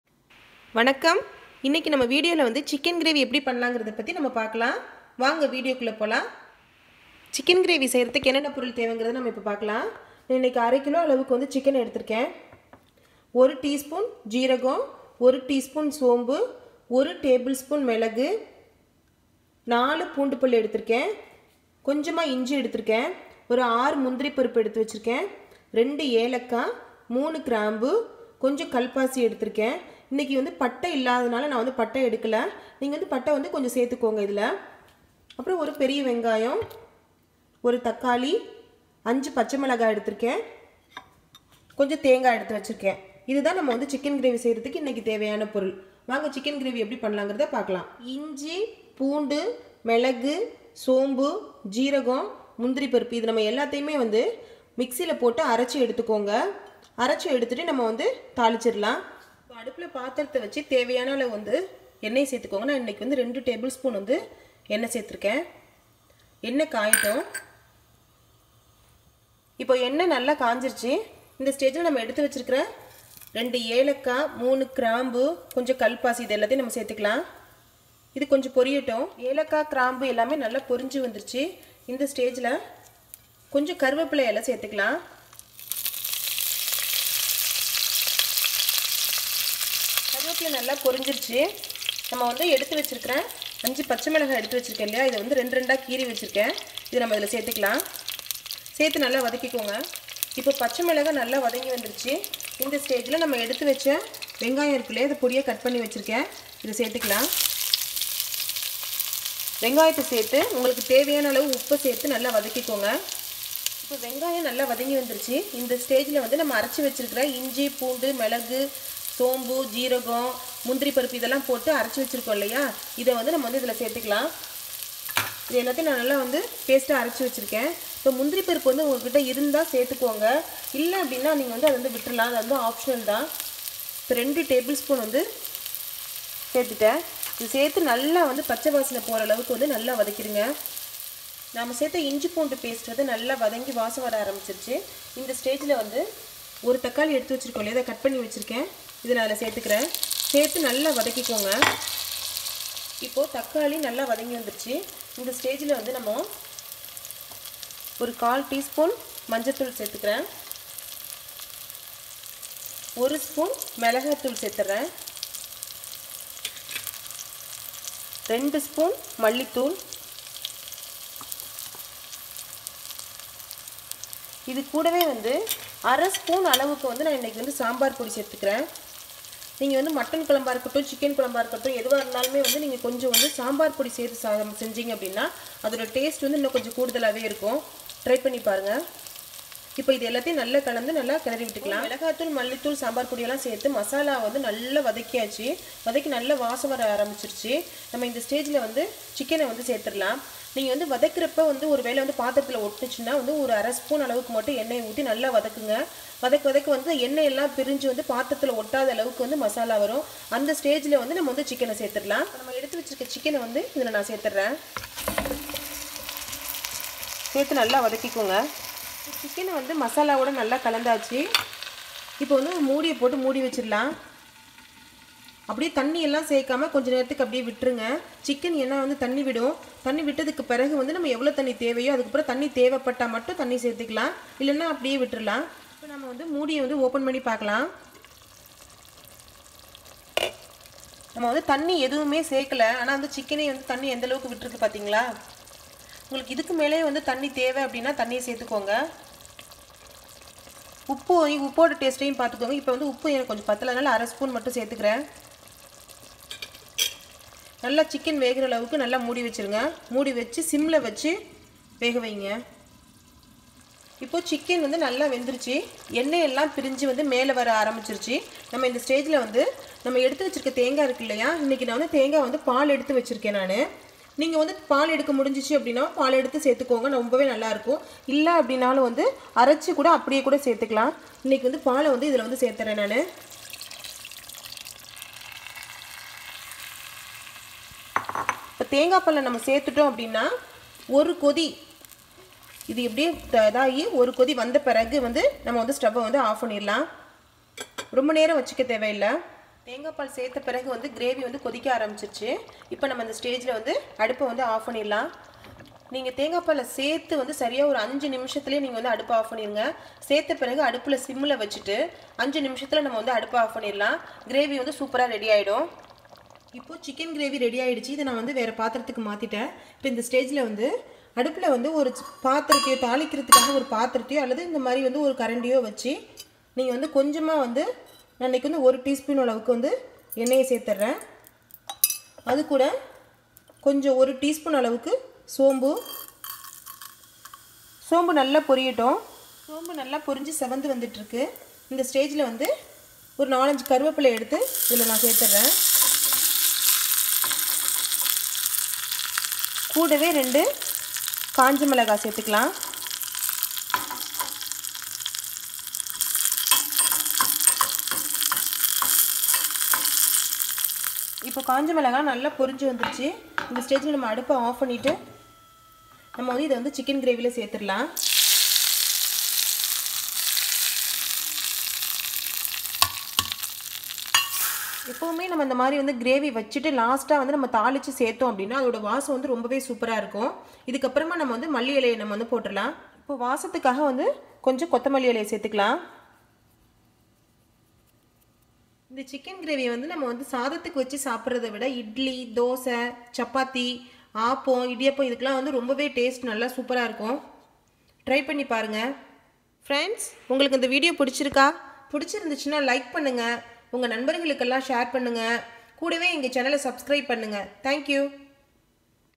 Thanjavur Special Chicken Gravy Recipe In Tamil/How to make chicken Curry In Tamil
Chicken gravy recipe in Tamil
      In this video we will see how to make chicken gravy in Tamil ,this recipe suitable for appam,idiyappam,chapathi and rice.taste is very good 😋